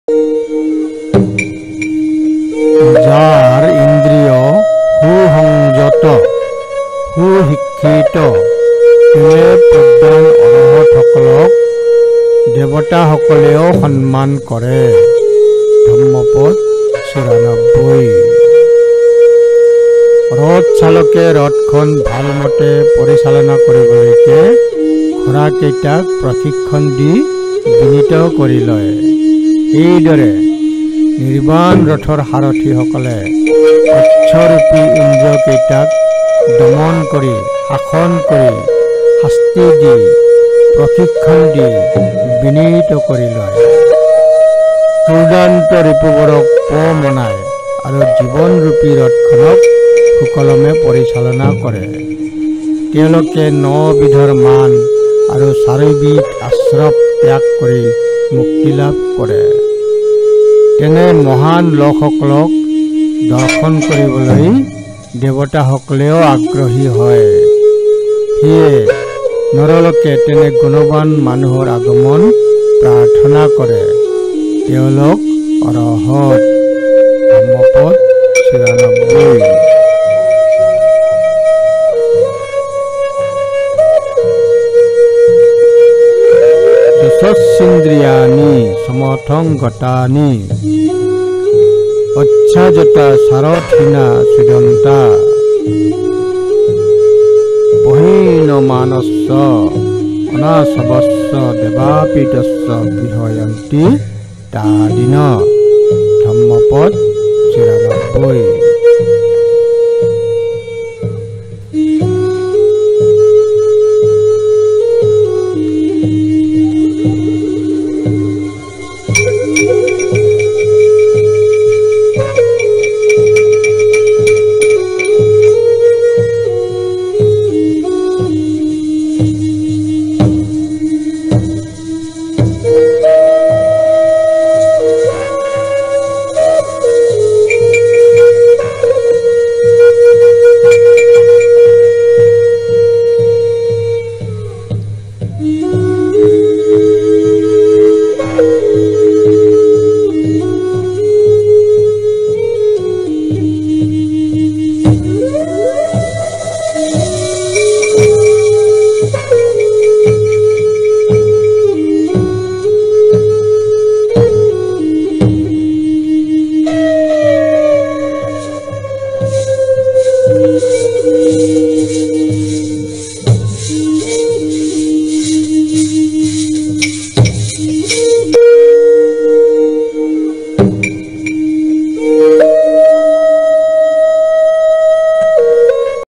Dhammapada-94,PPT in Assamese by D.Roy
D.P- 94. Vagga-7. Arahant Vagga. (90-99). Buddha Educational Foundation, Assam. Explained and animated by Durgeswar Roy. Treasury Of Truth: Illustrated Dhammapada. by Ven. Weragoda Sarada Maha Thero. All of the Buddha's teachings have been grouped into three collections or Three Baskets (Tripitakas). The three Pitakas are Vinaya, Abhidhamma, and Sutta. The present work Dhammapada is the second Book of the Minor Collection (Khuddakagama) of the Sutta Pitaka (The Basket of Discourses). It consists of 423 stanzas arranged in 26 Vaggas or Chapters. By reading the Dhammapada, one could learn the fundamentals of the Buddhist way of life.